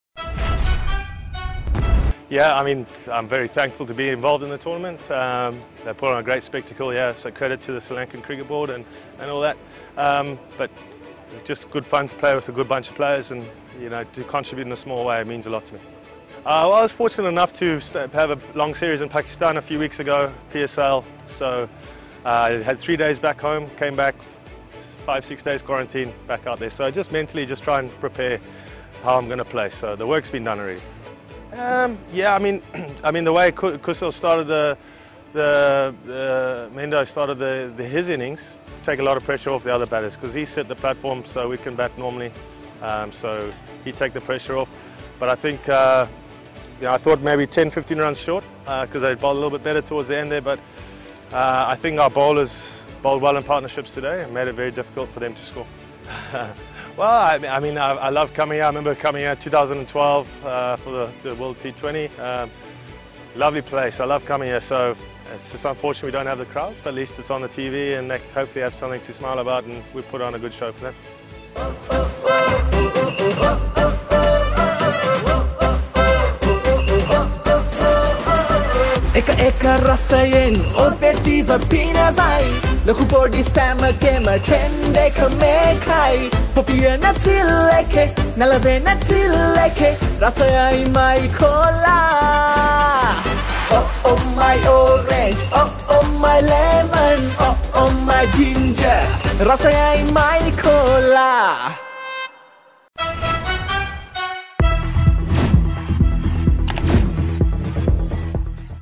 "Kusal Mendis set the platform for us to bat normally" - Brendon Taylor - LPL 2020 | My Cola
Kandy Tuskers’ middle order batsman Brendon Taylor explained the reasons for their match winning performance against Galle Gladiators